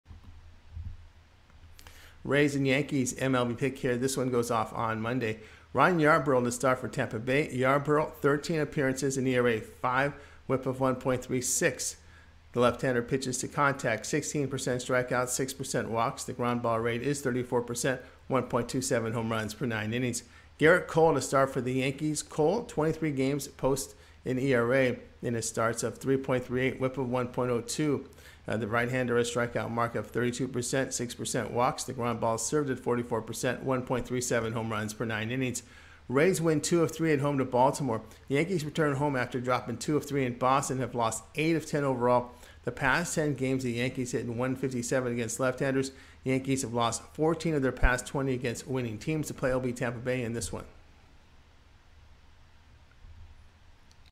Tampa Bay Rays vs. New York Yankees 8/15/2022 FREE MLB Picks and Predictions on MLB Betting Tips
8/15/2022 Tampa Bay Rays vs. New York Yankees FREE MLB Picks and Predictions on MLB Betting Tips for Today